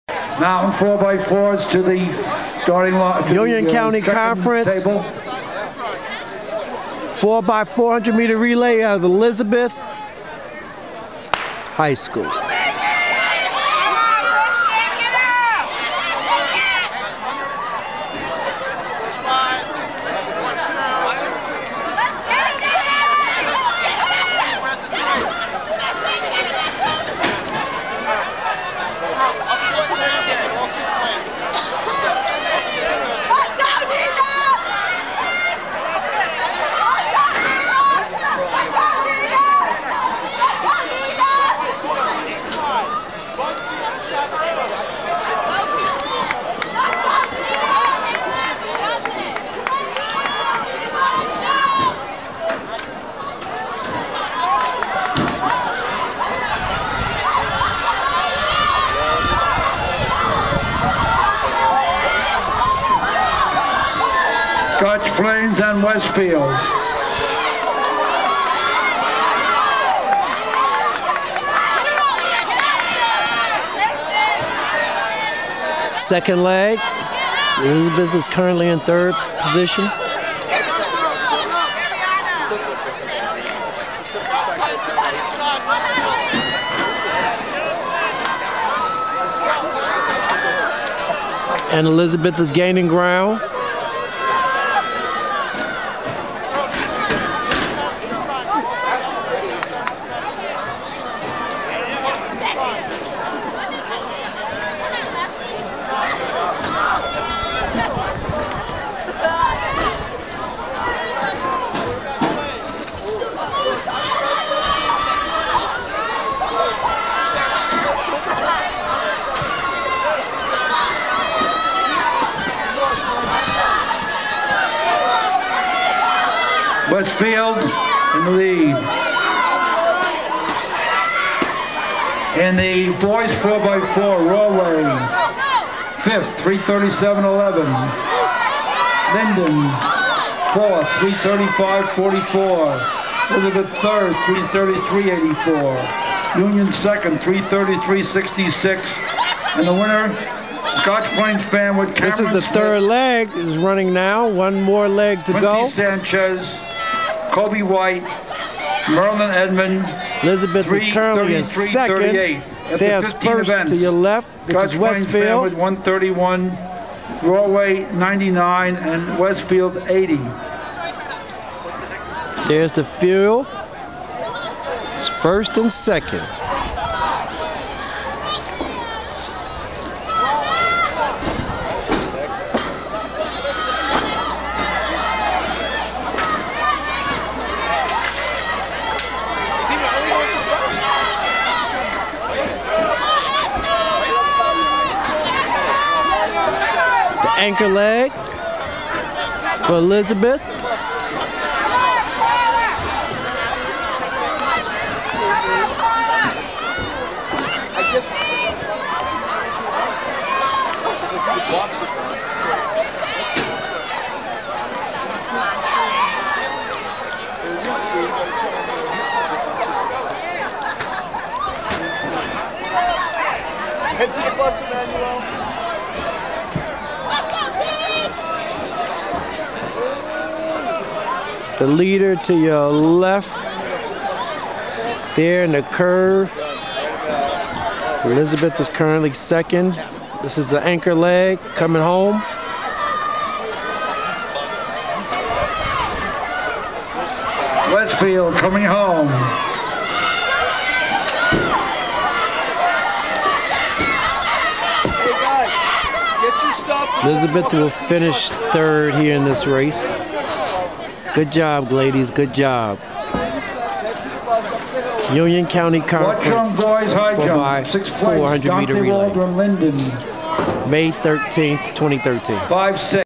UCCC,5-13-13,Elizth,Wtch-Div.4x400mRly,3rd.,Gior,Tamo,Nata,Esth-4.10.42
Union County Conference Championships, May 13, 2013, Elizabeth High School, Watchung Division, 4x400 Meter Relay, 3rd. Giorvania, Tamoya, Natalie, Ester - 4:10.42